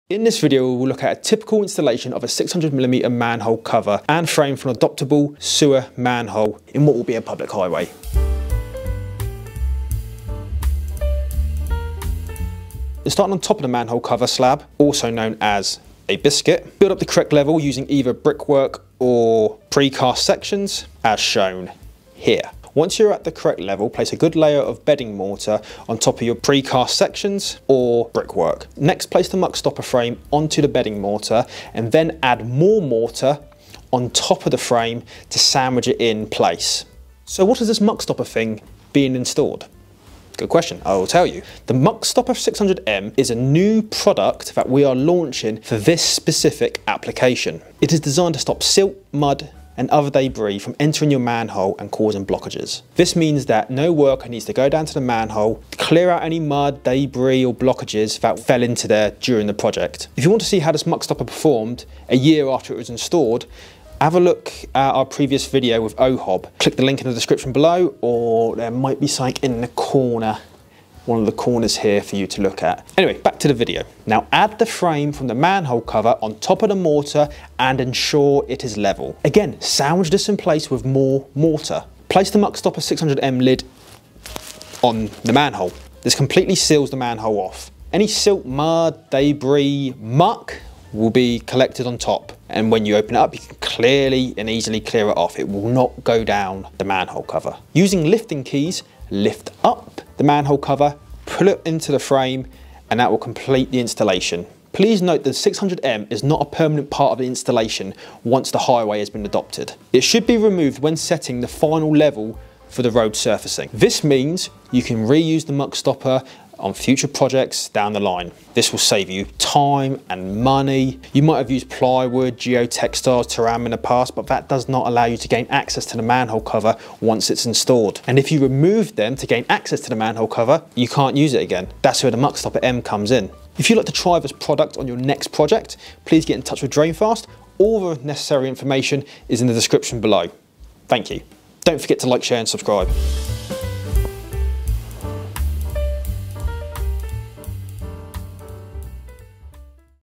How to install a Manhole Cover and Frame and MuckStopper 600M - Road and Sewer Construction
In this video, we will be detailing how to install an adoptable 600mm manhole cover and frame and the MuckStopper 600M.  We have designed the MuckStopper 600M to solve the challenge of mud and silt entering 600mm manhole covers and clogging the drainage system below. This watertight barrier protects the drains while serving as a secondary manhole cover.

0:00 - Introduction
0:12 - Install precast concrete sections or brickwork
0:28 - Install MuckStopper 600M frame
0:37 - What is the MuckStopper 600M?
1:13 - Install manhole cover frame
1:37 - Install manhole cover using lifting keys
1:44 - Important information about the MuckStopper 600M
2:01 - MuckStopper Alternatives
2:20 - Ending

For more information on the MuckStopper 600 please check out our

For more information regarding the MuckStopper range please click this